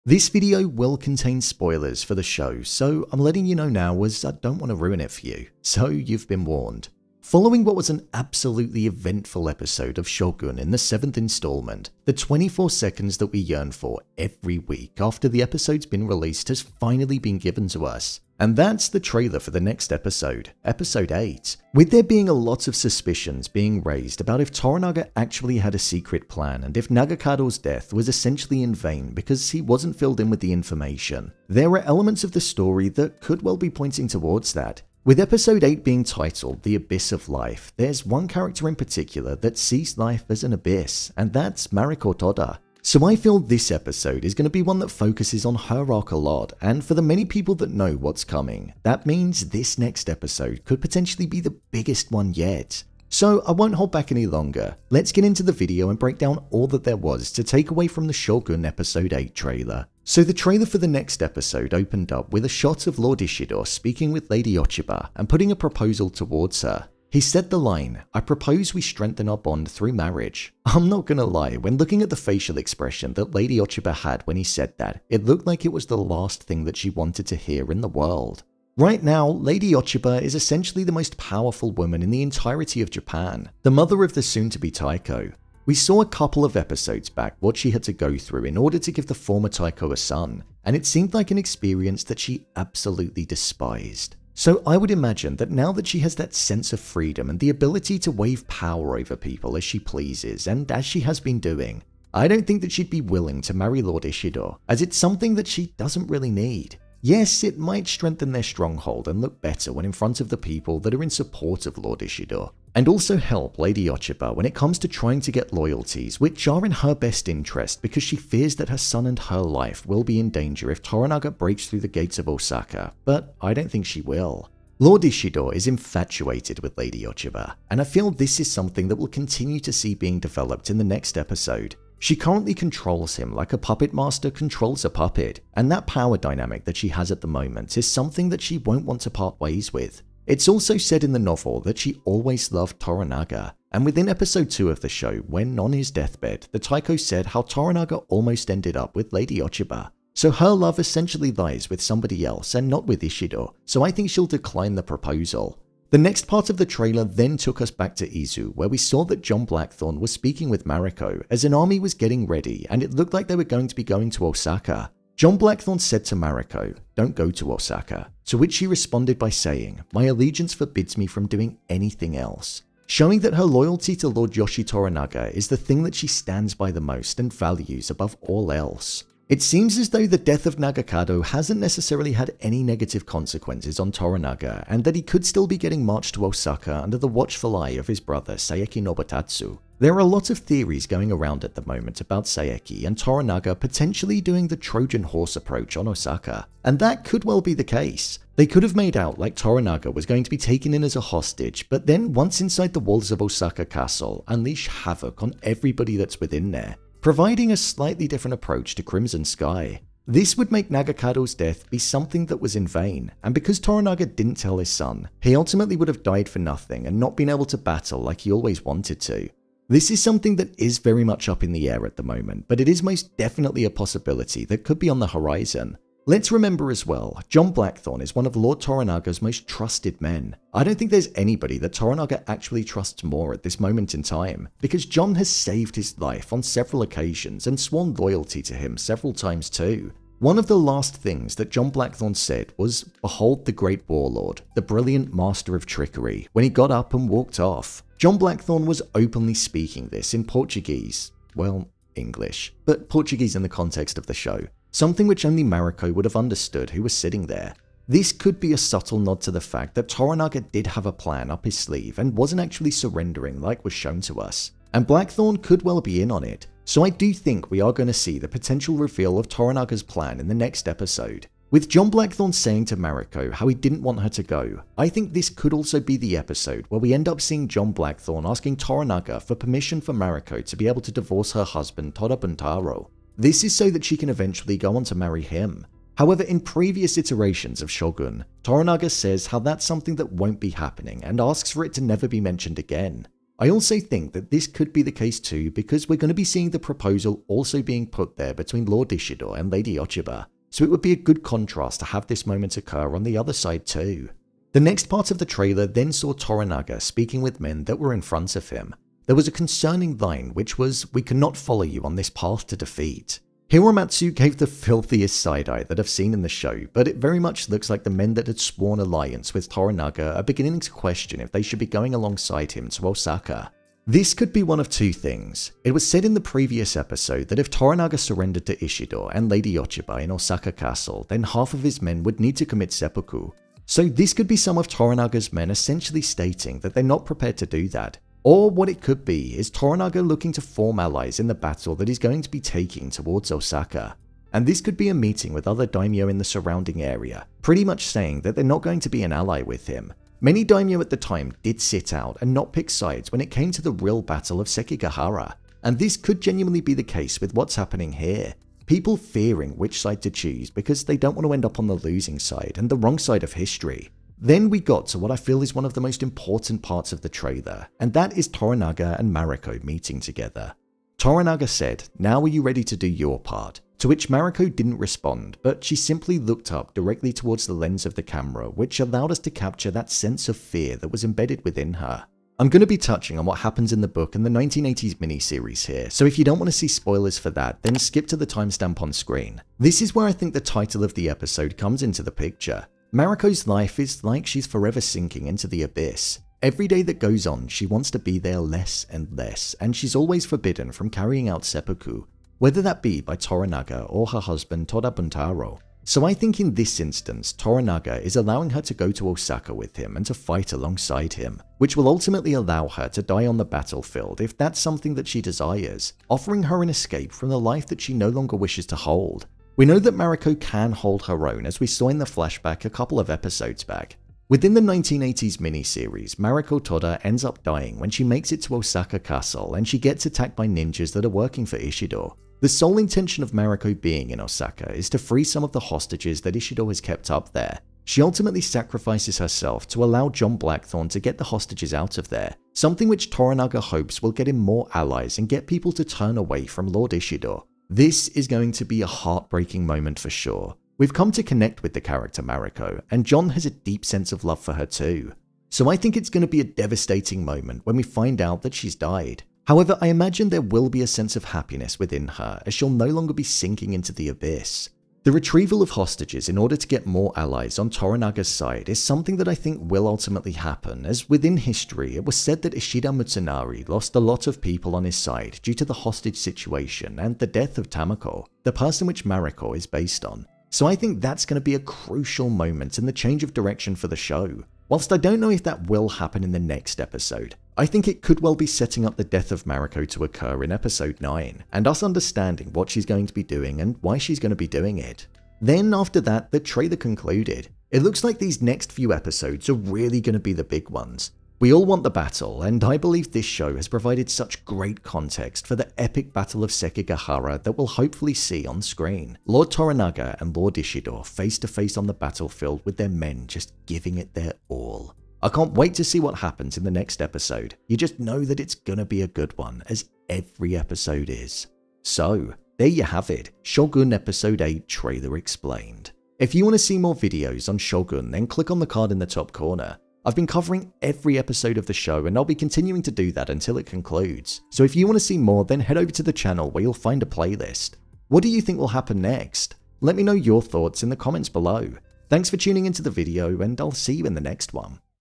SHOGUN Episode 8 Trailer Explained
I review, breakdown and explain the Shogun Episode 8 Trailer. I react to the preview which is focussed on Lord Ishido looking to marry Lady Ochiba, Mariko’s death, and Toranaga's secret plan of going to Osaka in the buildup to the battle of Sekigahara following the death of his son Nagakado. I also give my theories and predictions on episode 8.

00:00 Intro
00:59 Shogun Episode 8 Trailer Breakdown
08:41 Outro